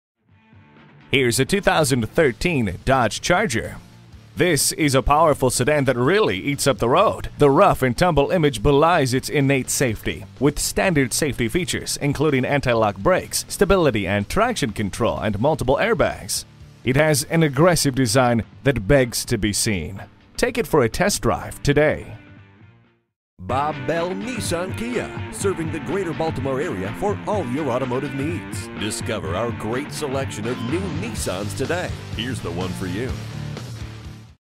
2013 Dodge Charger Baltimore MD Dundalk, MD 22774Z - SOLD
Year: 2013
Make: Dodge
Model: Charger
Trim: SE
Engine: 3.6L V6
Transmission: Automatic 5-Speed
Color: Pitch Black
Interior: Black Cloth
Mileage: 22695
Stock #: 22774Z
One-Owner Carfax. 22-Inch Chrome Wheels & Performance Tires Just Added! Pitch Black 2013 Charger SE. Keyless Ignition Dual Zone Air Multi-Function Remote Power Adjustable Driver's Seat and more! Antimicrobial Interior Protection. Free Oil Changes! Lifetime BellCare!

7900 Eastern Ave.

Address:
7900 Eastern Ave